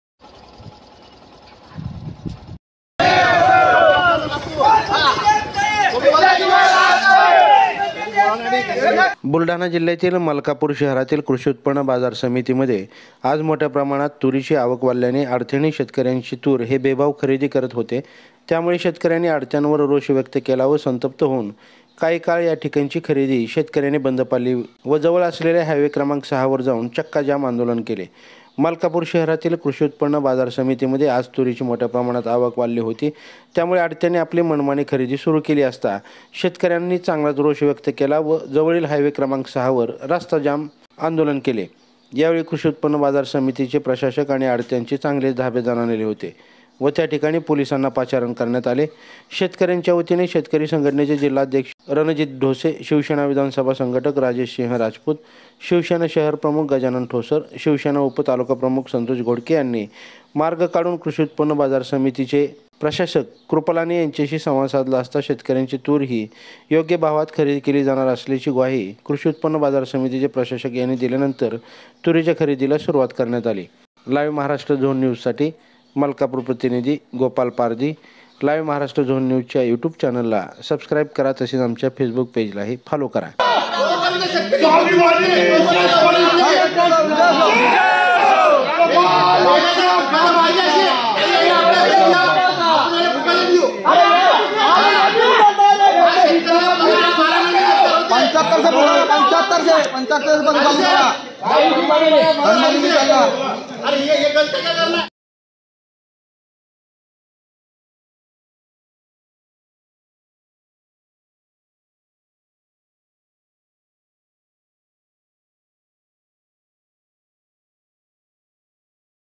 कृषी उत्पन्न बाजार समितीत 'राडा'शेतकऱ्यांनी सौदे बंद पाडलेबाजार
LiveMaharashtraZone News